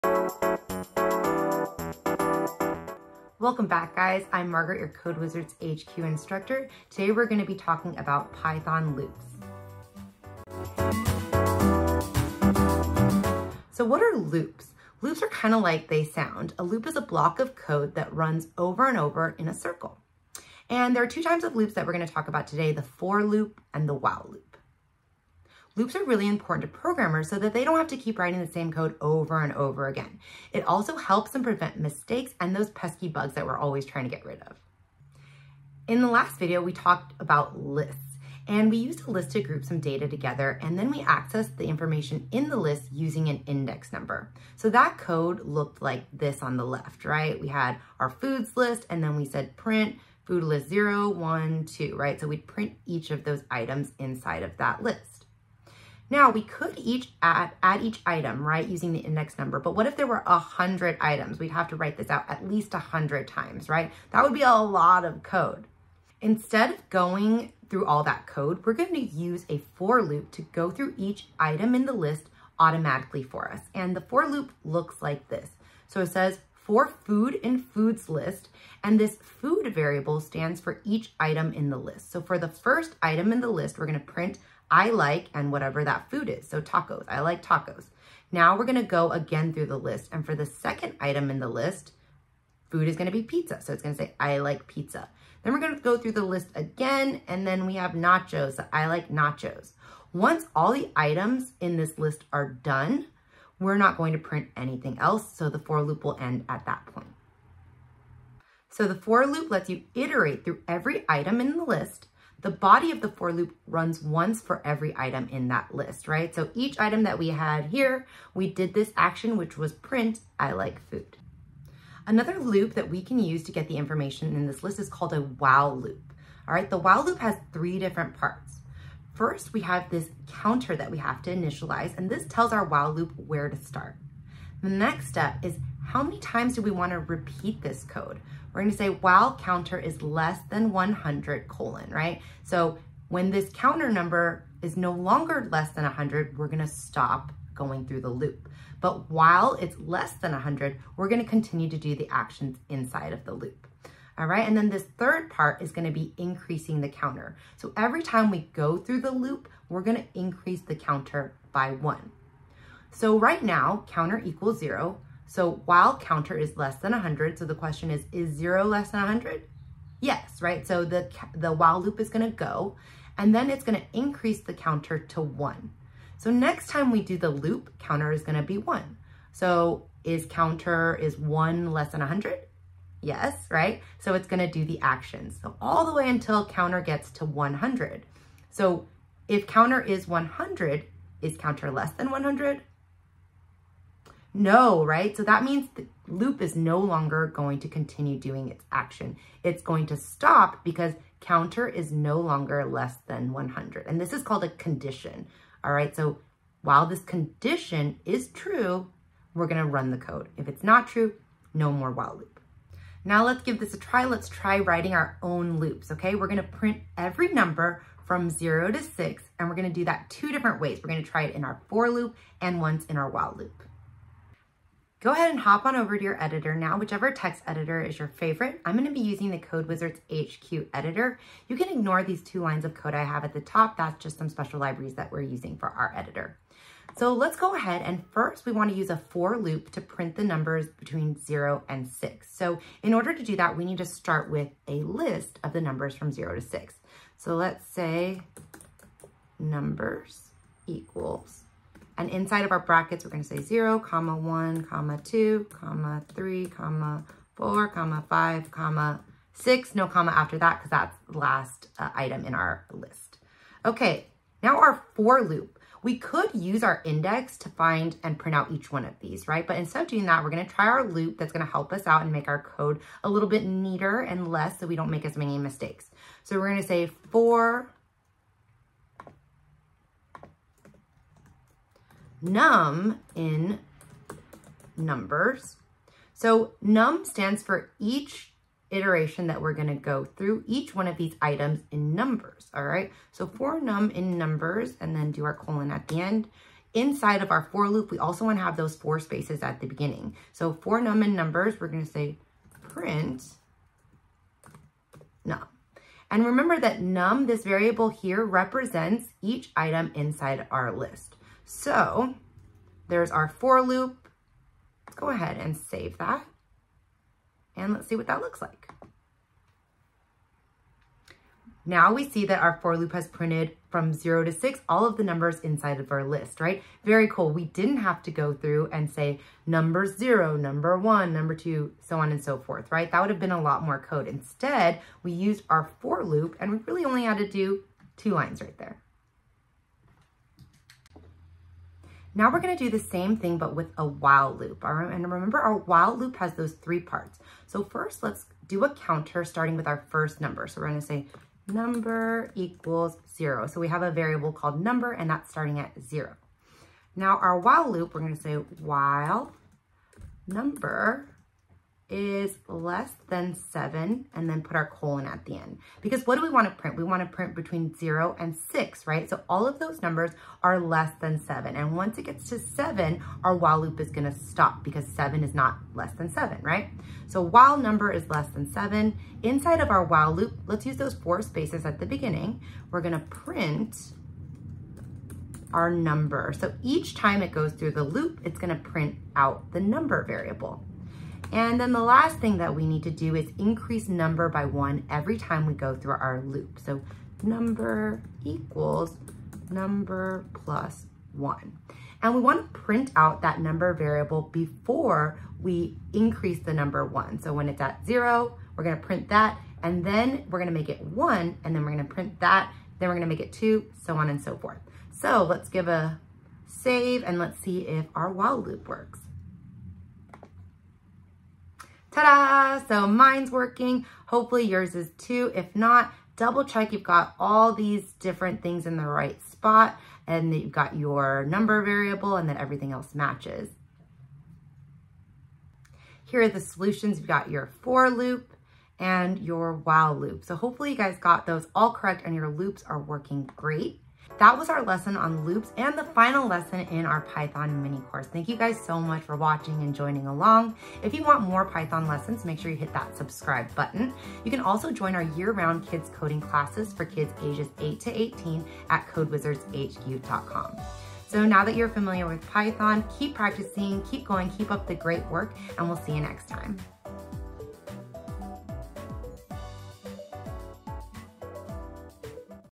Python Loops - Intro to Python for Kids
A Python loop is a block of code that runs over and over while a certain condition is met.

In this tutorial, I show kids how to write a for and while loop in Python and explain why loops are so important to programmers.

Join the most fun and effective online Python classes for kids